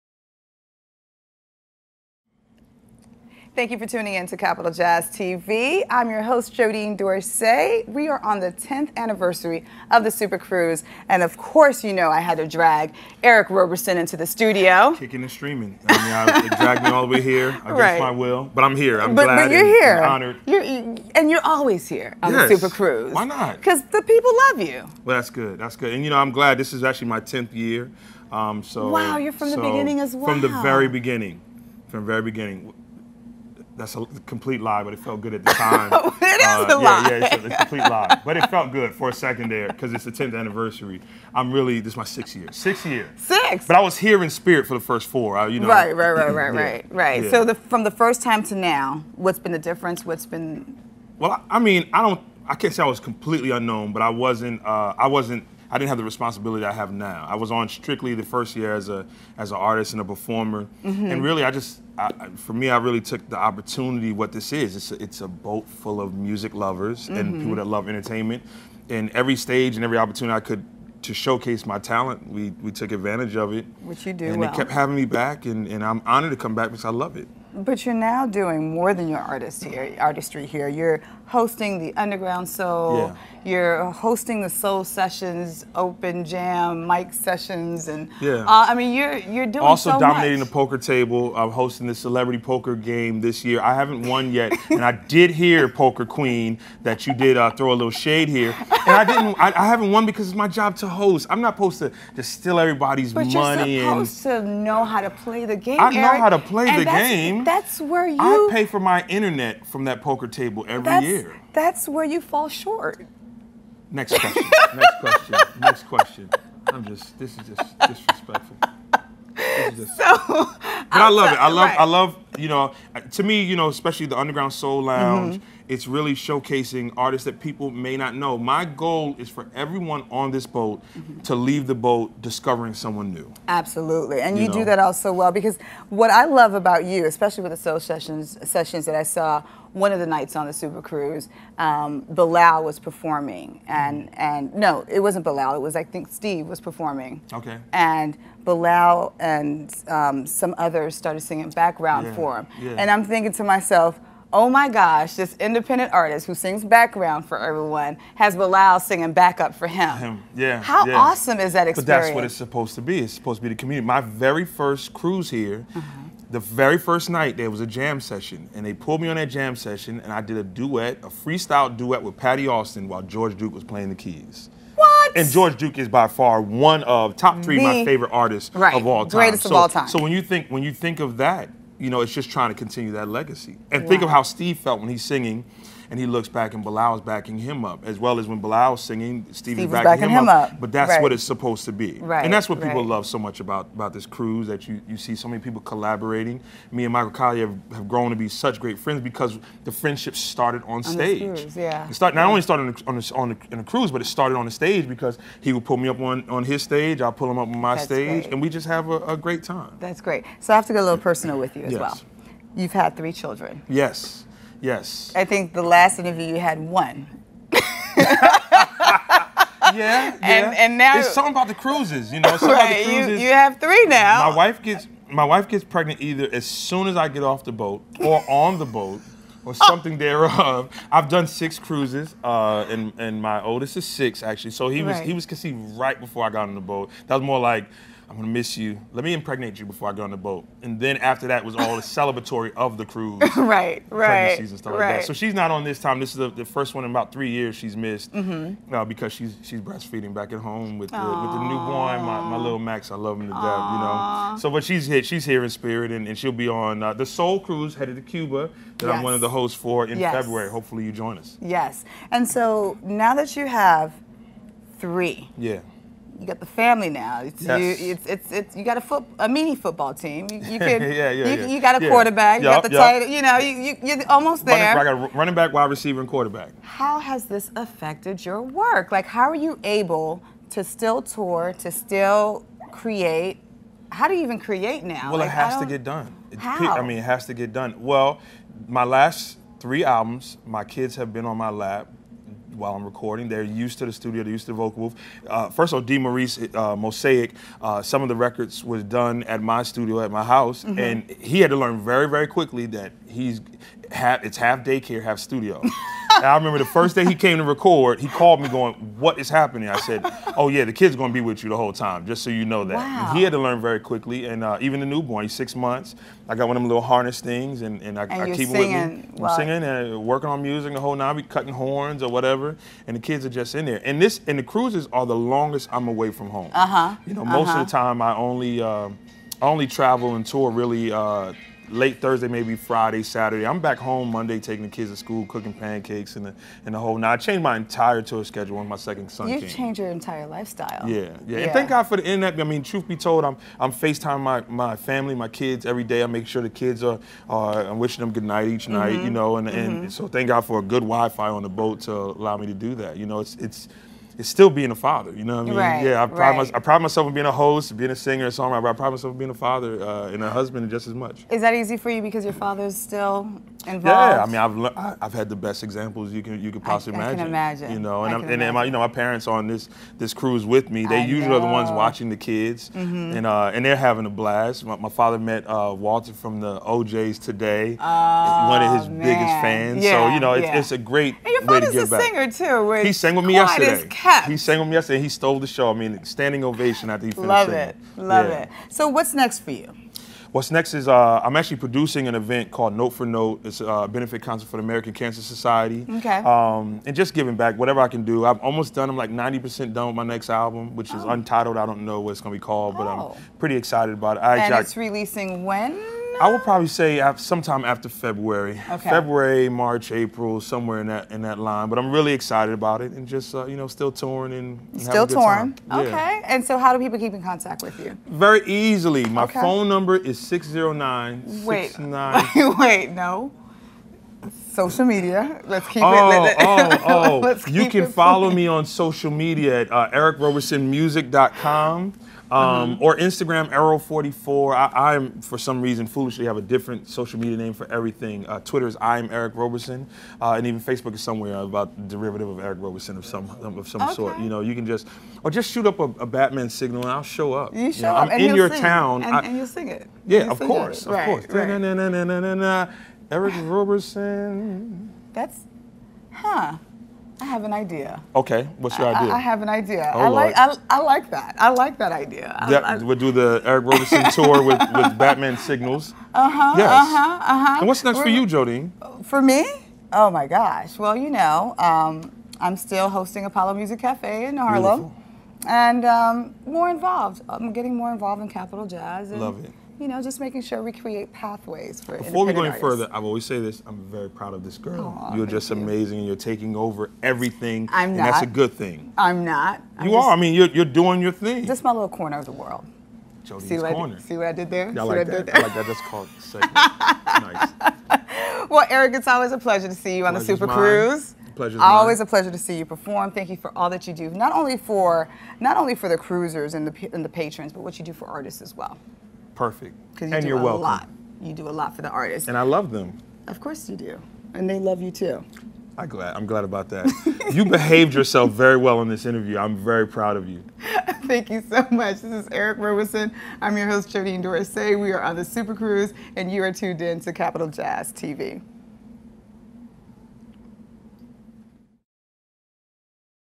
Eric Roberson - Capital Jazz TV interview from The SuperCruise X
Interview with musical host Eric Roberson during our 10th annual Capital Jazz SuperCruise, October 2016. See this and other behind-the-scenes video footage on our Capital Jazz TV channel